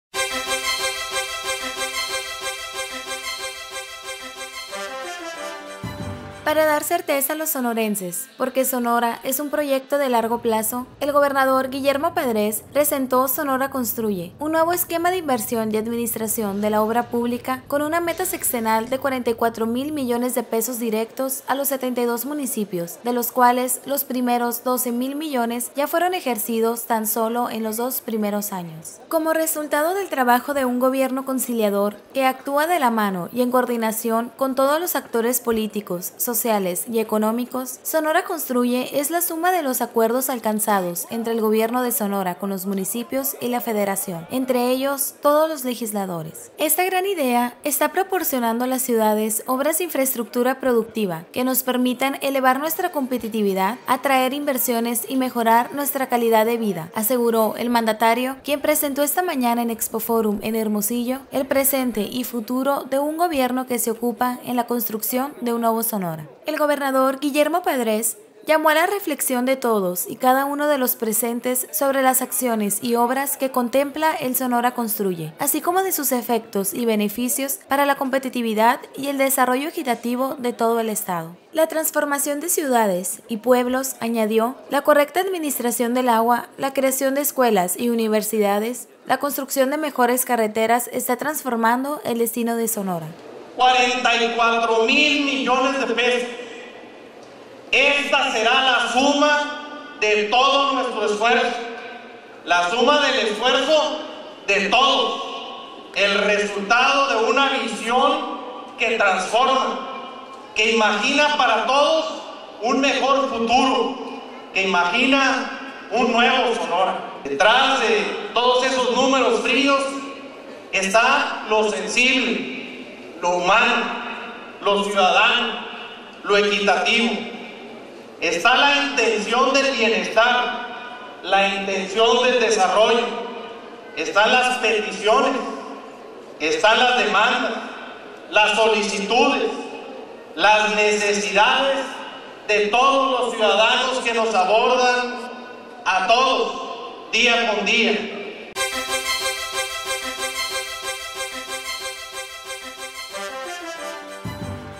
Gobernador Guillermo Padrés presentó proyecto "Sonora Construye". 08-08-2011
Hermosillo, Sonora. Para dar certeza a los sonorenses porque Sonora es un proyecto de largo plazo,  el Gobernador Guillermo Padrés presentó “Sonora Construye”, un nuevo esquema de inversión y administración de la obra pública con una meta sexenal de 44 mil millones de pesos directos a los 72 municipios, de los cuales los primeros 12 mil millones ya fueron ejercidos tan sólo en los dos primeros años.

Fecha del evento: 08 de Agosto de 2011